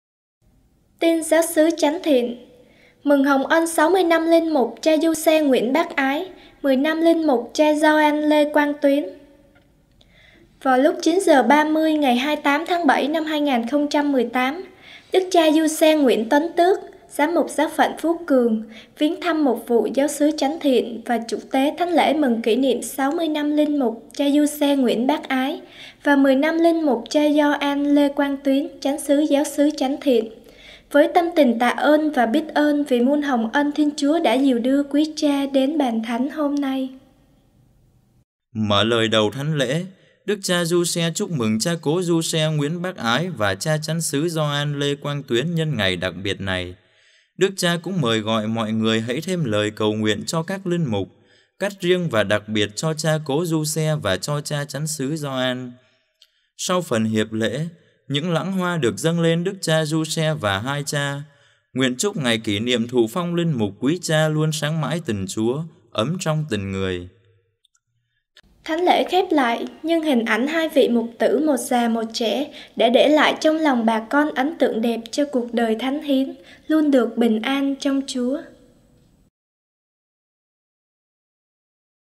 Mừng Hồng Ân 60 Năm Linh Mục Cha Giuse Nguyễn Bác Ái 10 Năm Linh Mục Cha Gioan Lê Quang Tuyến
Trích: Điểm Tin Giáo Phận Phú Cường Tuần Qua: 30.7 đến 4.8.2018